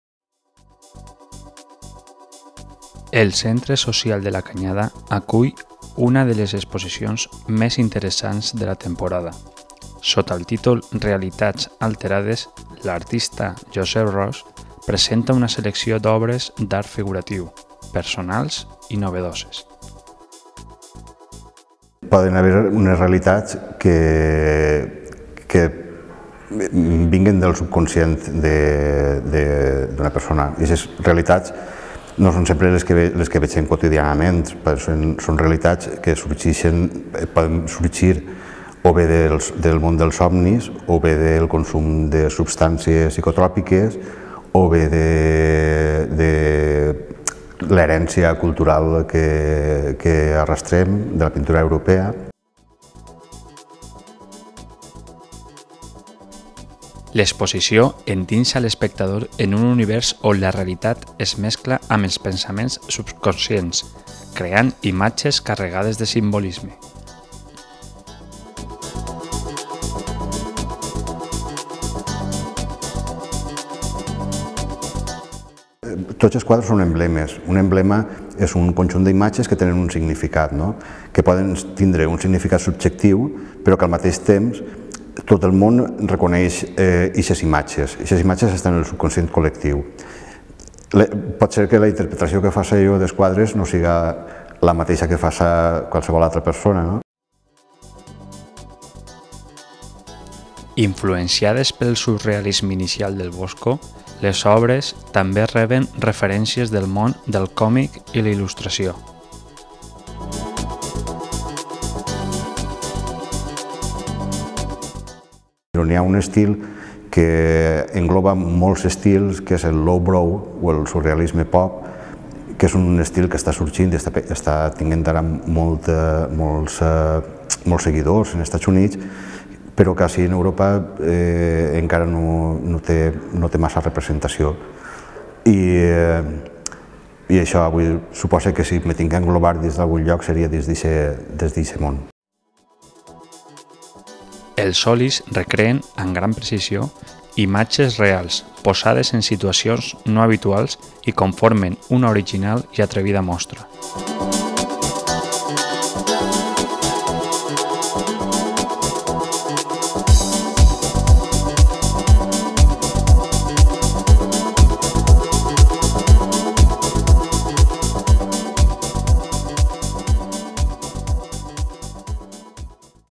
"REALITATS ALTERADES" de Josep Ros
Descripción: El Centro Social de la Canyada acoge una de las exposiciones más interesantes de la temporada. El artista Josep Ros nos presenta una selección de obras de arte figurativo,  personales y novedosas.
La exposición “Realitats Alterades”, adentra al espectador en un universo donde la realidad se mezcla con los pensamientos subconscientes creando imágenes cargadas de simbolismo. Influenciadas por el surrealismo inicial de El Bosco, las obras también reciben referencias del mundo del cómic y la ilustración. Los óleos recrean, con gran precisión, imágenes reales puestas en situaciones no habituales y conforman una original y atrevida muestra.
La muestra se inauguró el pasado 16 de enero y estará abierta al público hasta el próximo 20 de febrero.